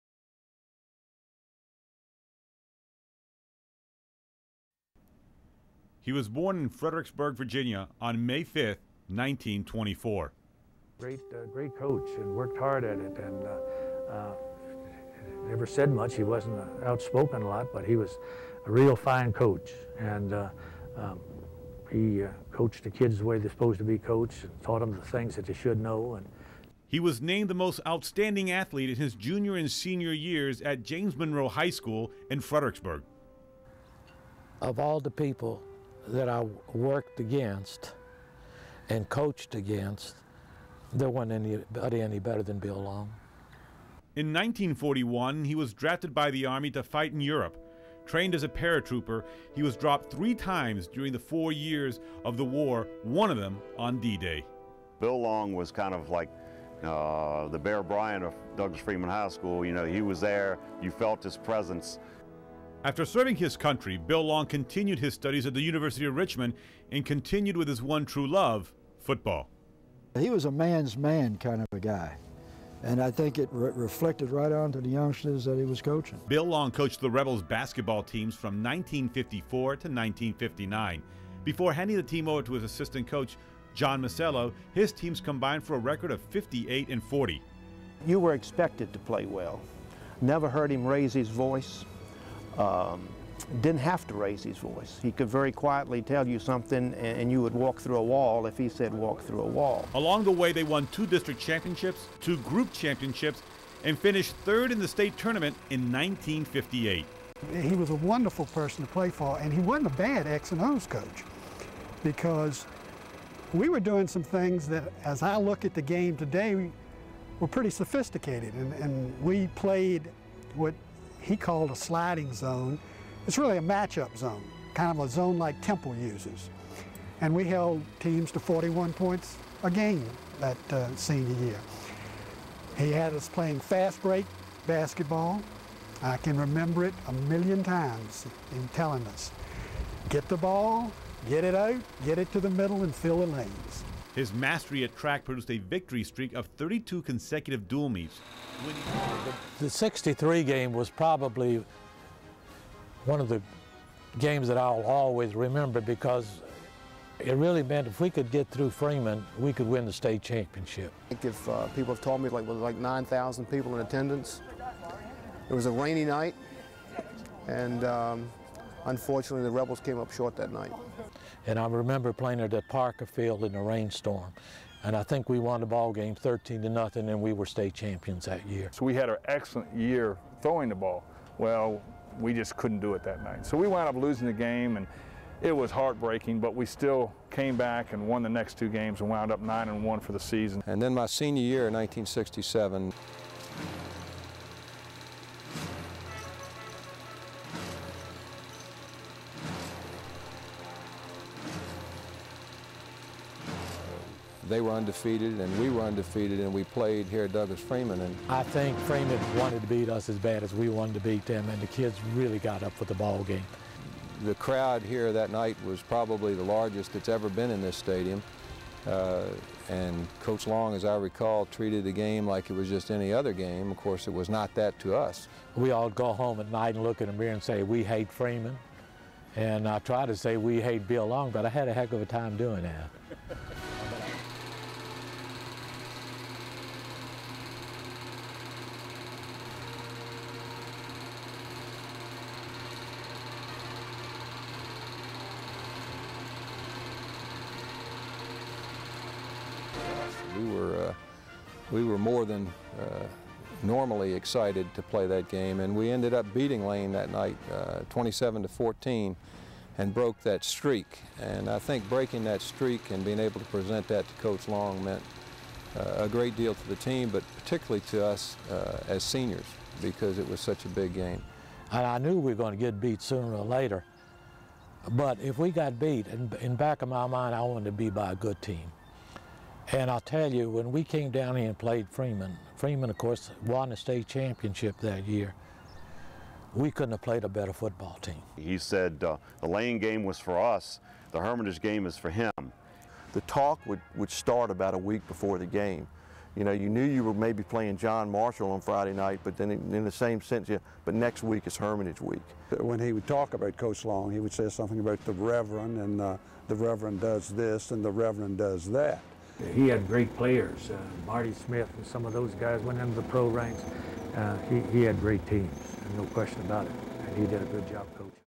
Documentary - Bill Long.mov
Documentary of Freeman High School ( Richmond, VA.) football coach Bill Long. Produced for Henrico County Public Schools.